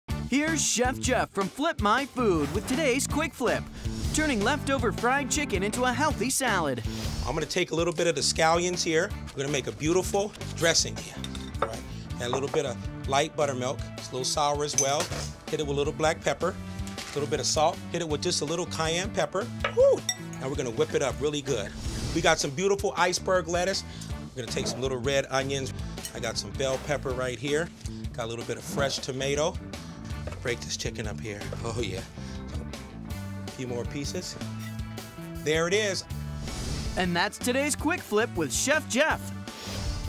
Fried Chicken Salad
Get the full list of ingredients here:

Ingredients
• Leftover fried chicken
• 1 tablespoon scallions
• 2 tablespoons buttermilk
• 1 teaspoon black pepper
• 1 teaspoon salt
• ¼ tablespoon cayenne pepper
• 5 iceberg lettuce leaves
• 1 tablespoon chopped red onions
• ¼ bell pepper, sliced
• 1 tablespoon diced tomato

Method
− Whip together scallions, buttermilk, black pepper, salt, and cayenne pepper to make dressing
− Chop iceberg lettuce and fried chicken
− Toss together iceberg lettuce, fried chicken, red onions, bell pepper, and tomato
− Drizzle salad in dressing